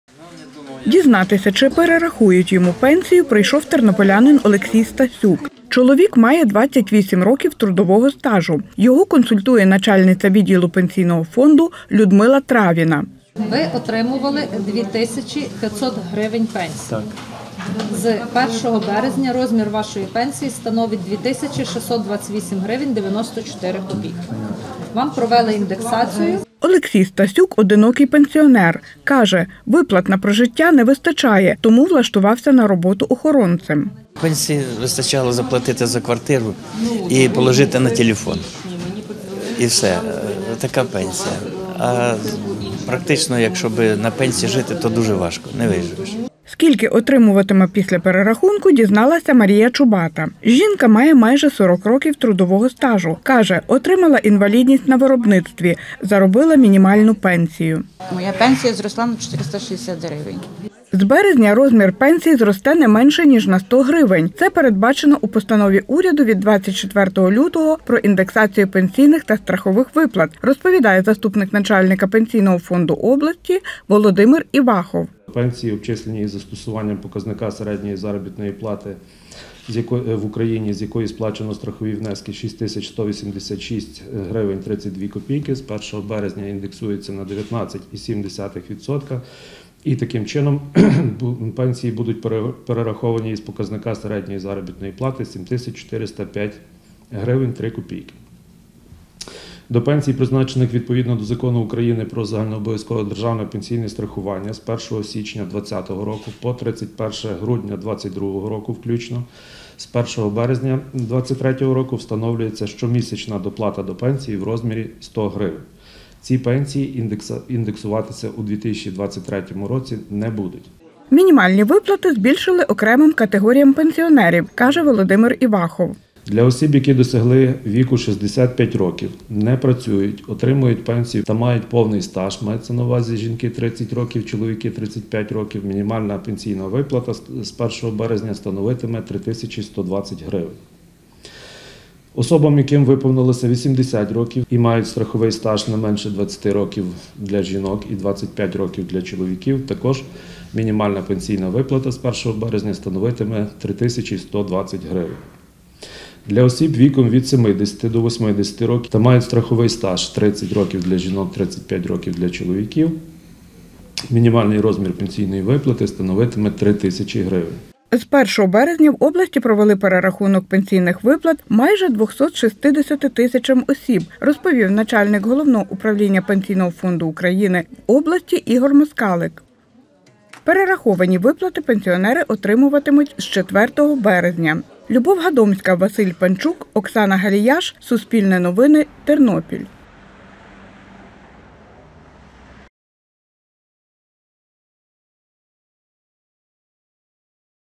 На Тернопільщині пенсіонерам перераховують пенсії: хто отримає доплати
З 1 березня в області почали перераховувати пенсійні виплати. Мінімально вони зросли на 100 гривень. Перераховані виплати пенсіонери будуть отримувати з 4 березня. Про це розповів Суспільному начальник Головного управління пенсійного фонду України в області Ігор Москалик.